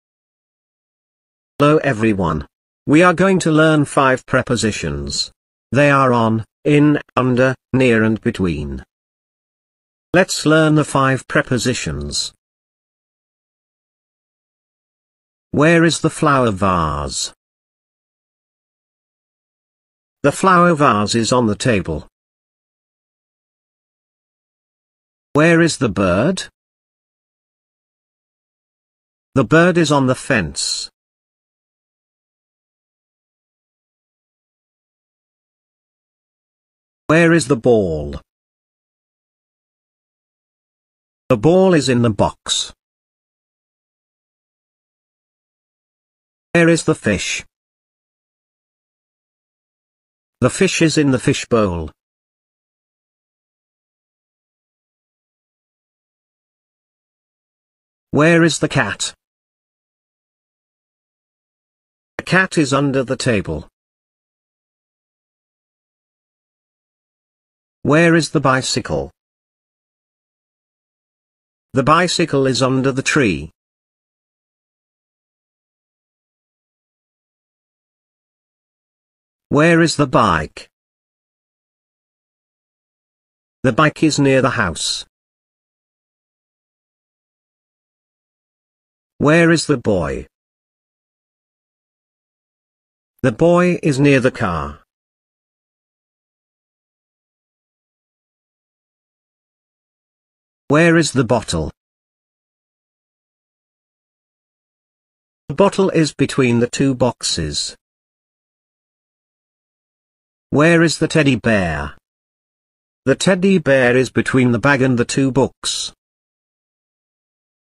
Prepositions || Learn Prepositions || English grammar prepositions
A preposition is a word or group of words used before a noun, pronoun, or noun phrase to show direction, time, place, location, spatial relationships, or to introduce an object. Some examples of prepositions are words like "in," "at," "on," "of," and "to."